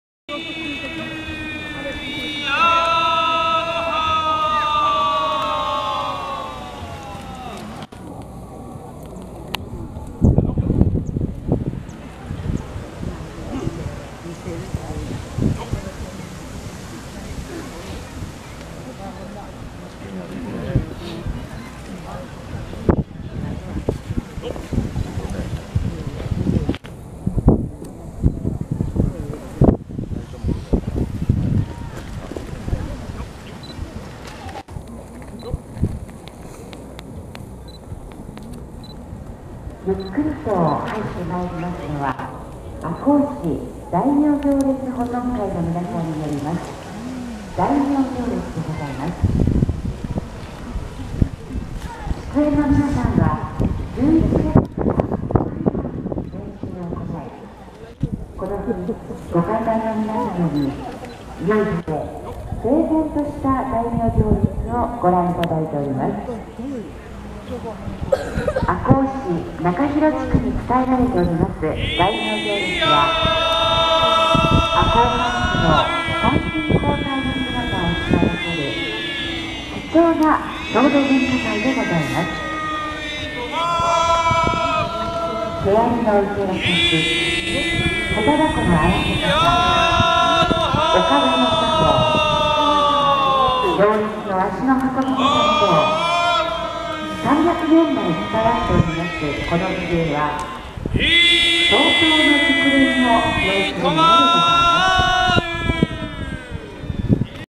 第１１１回赤穂義士祭『大名行列』
赤穂城跡で開催された第１１１回赤穂義士祭『大名行列』の模様です。